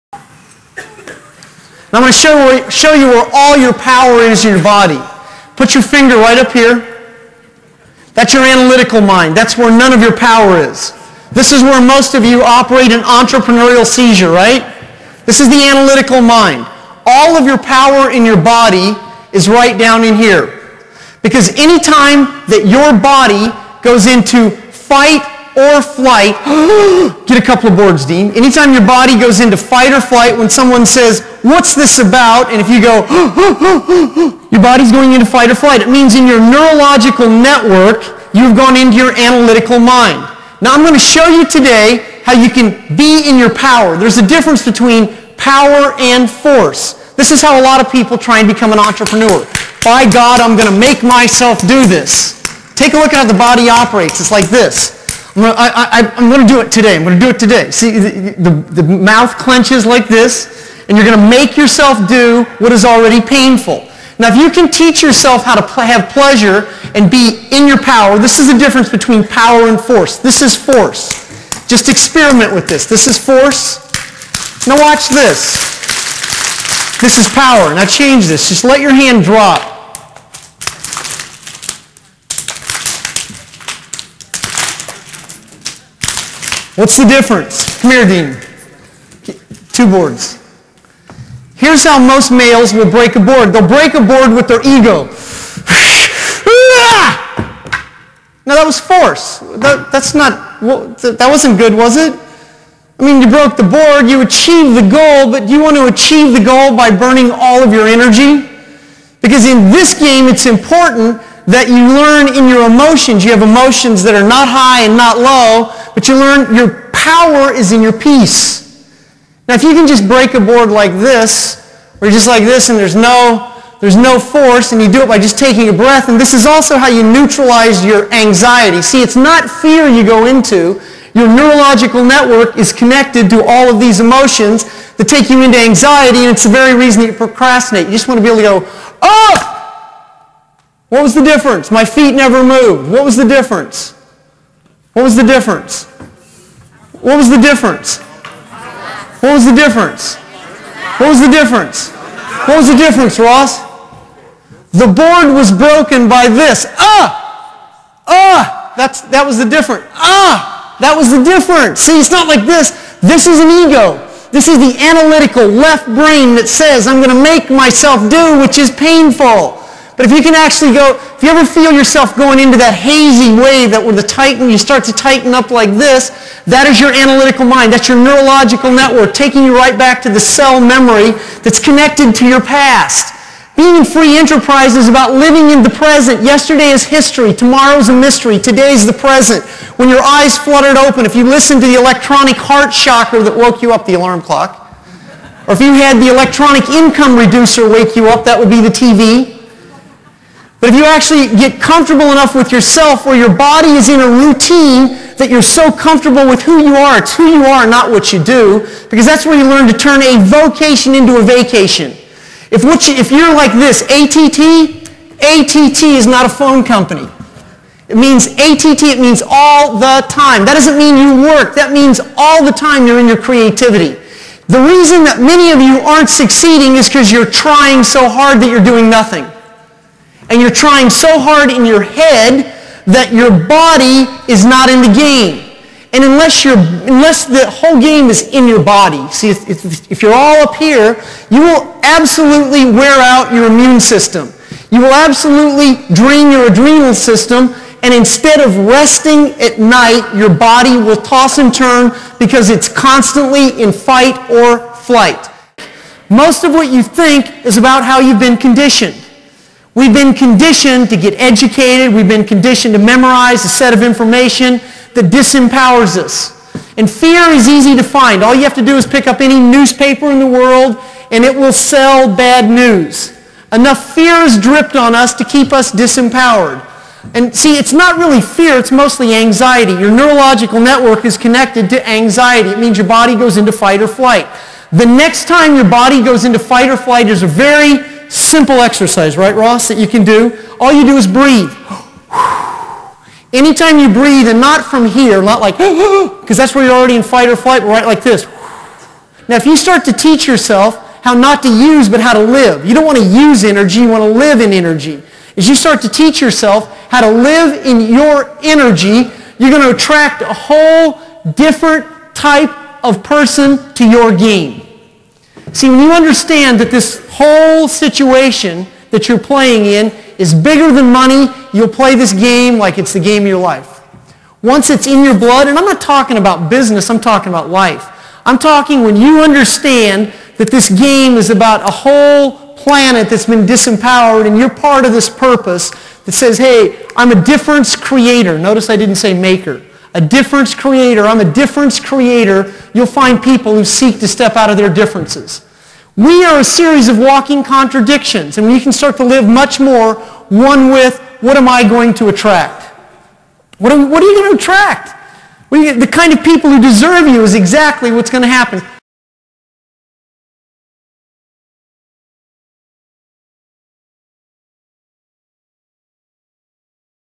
Jeff Combs - Empowerment, Abundance, & Utilizing Your Energy
Jeff Combs talks about creating abundance by using your power (spirit), not force (ego). Get out of your analytical mind and get into the flow of your energy. It is the power of presence and living in the now. It is our neurological networks that bring out our fear and anxiety and block the ease and flow of attaining our goals and desires. This relates to the law of attraction. We can be empowered in a disempowered world.